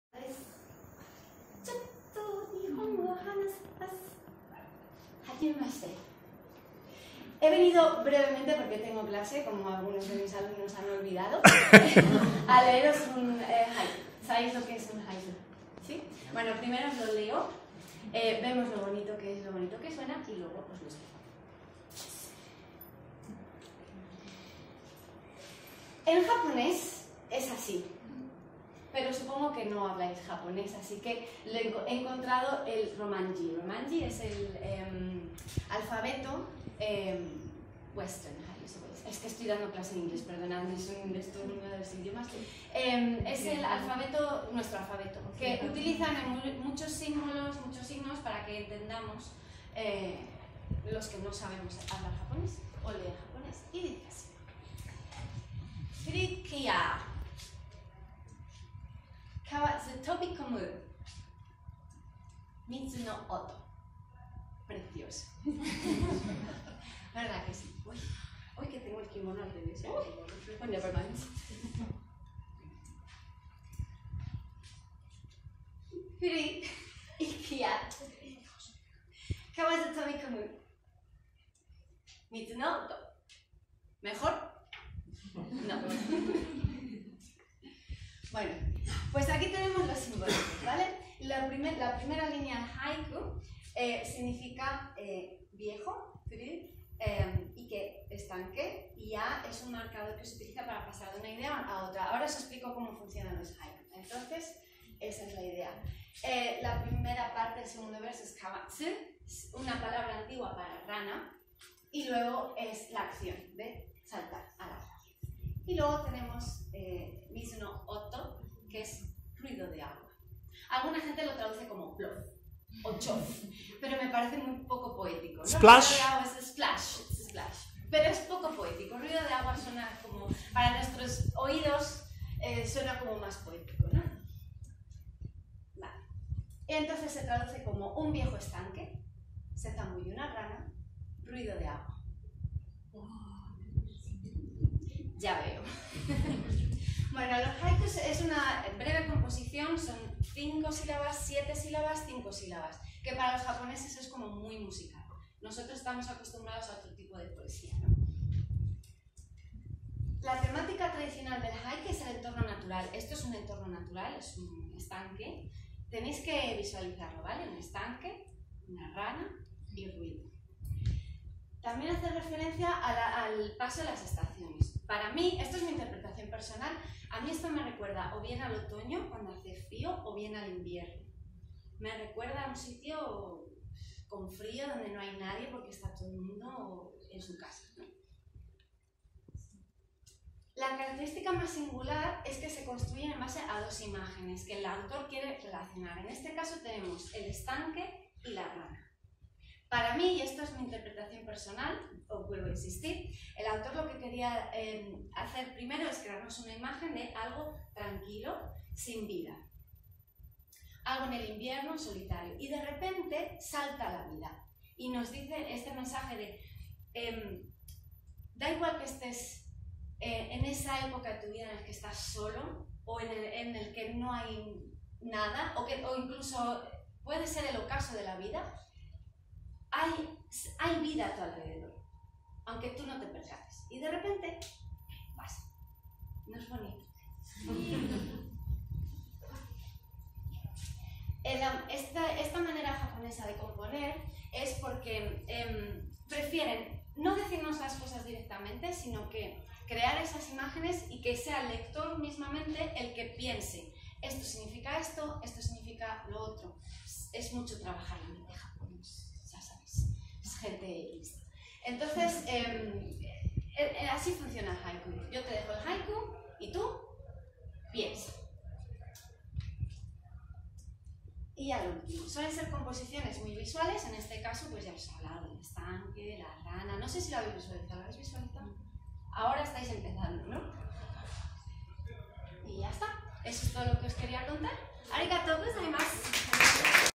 Japonés. Furu ike ya, de Matsuo Basho. Recita Lorena Alonso
Escuela Oficial de Idiomas de Valdemoro. Día Europeo de las lenguas. Septiembre 2018.